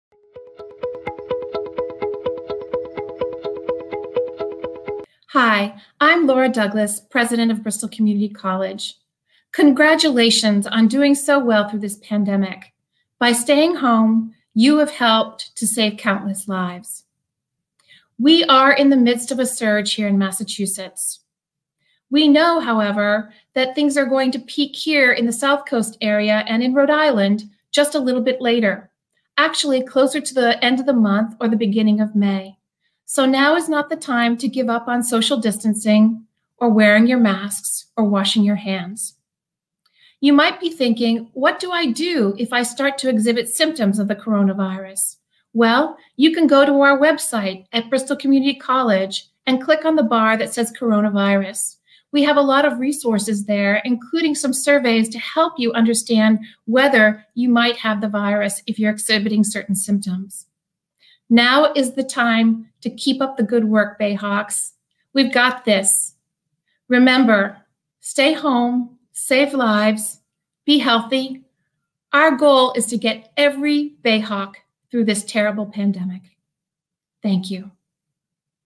A Message from Laura L. Douglas, Ph.D. - April 21, 2020
The latest from Bristol Community College President Dr. Laura Douglas on the college's response to COVID-19. April 21, 2020.